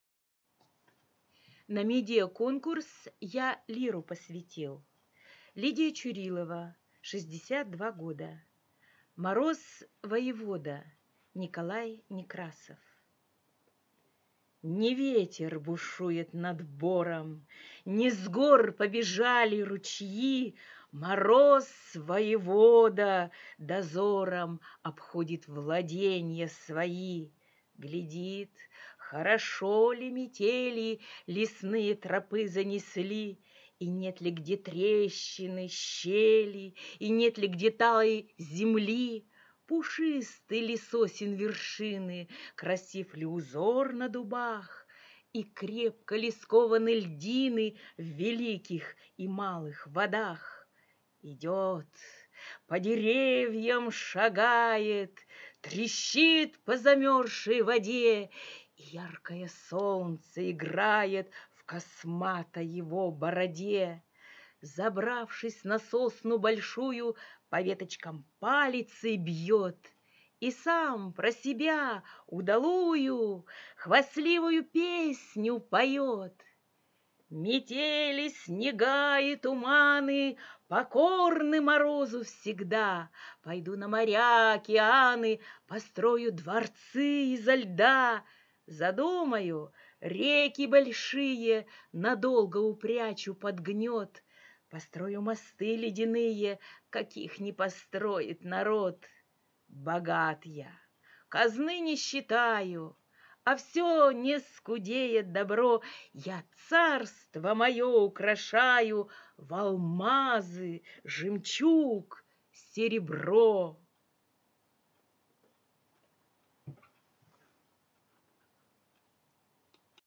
Чурилова Лидия. Отрывок из поэмы Н.А. Некрасова "Мороз, Красный нос" "Мороз-воевода"
На конкурс "Я лиру посвятил..." к 200-летию Н.А. Некрасова